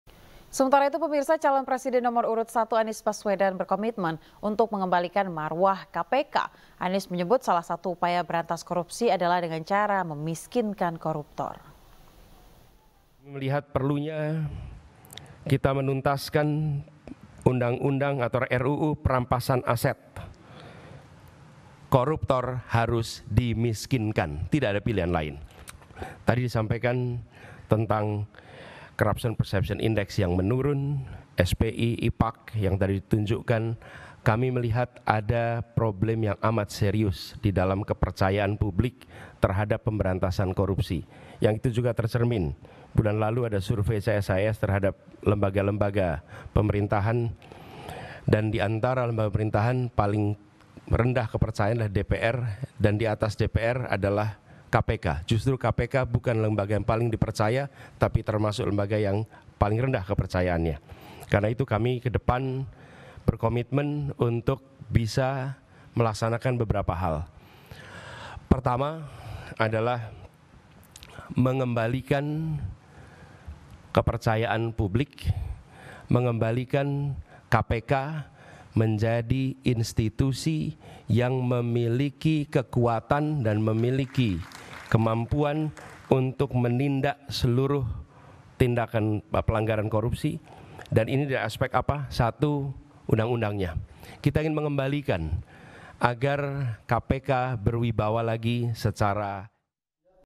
Anies Baswedan: Memiskinkan Koruptor Cara Ampuh Berantas Korupsi
Anies Baswedan: Memiskinkan Koruptor Cara Ampus Berantas Korupsi